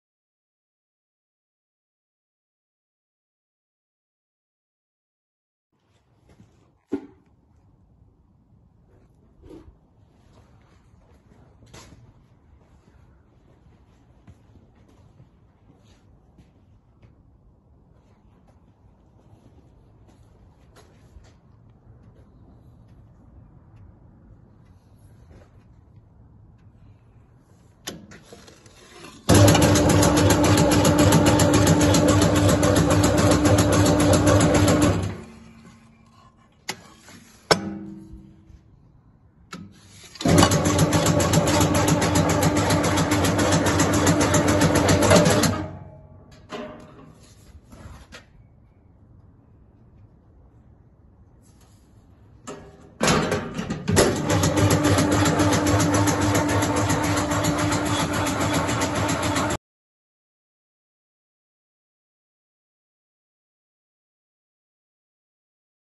1125-77: Rühle dicer
Rühle dicer, model: MR 80, dimensions chamber: 300 mm x 100 mm x 100 mm, total dimensions: 1100 mm x 600 mm x 1000 mm.